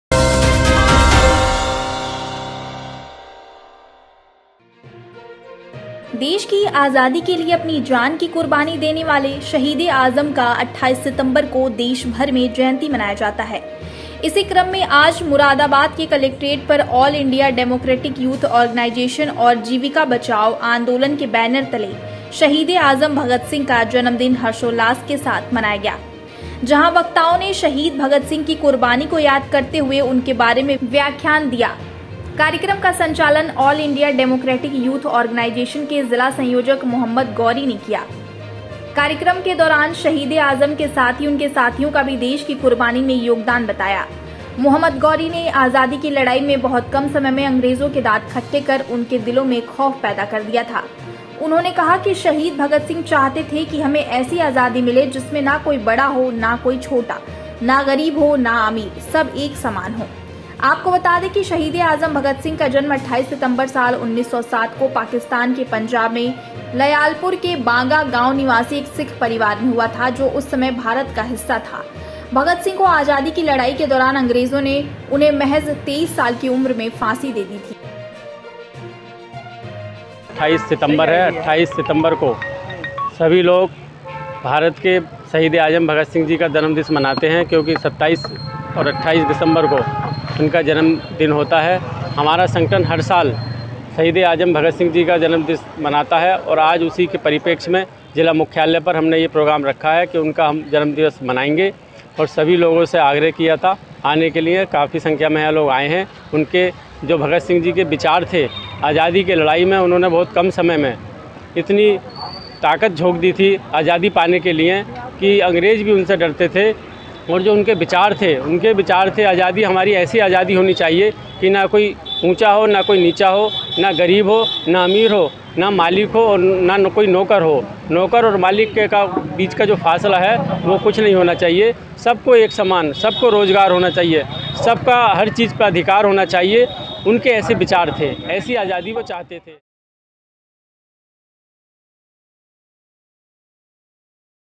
मुरादाबाद: शहीद ए आजम की कुर्बानी को किया याद, भगत सिंह की मनाई 115वीं जयंती | Amritvichar
मुरादाबाद: शहीद ए आजम की कुर्बानी को किया याद, भगत सिंह की मनाई 115वीं जयंती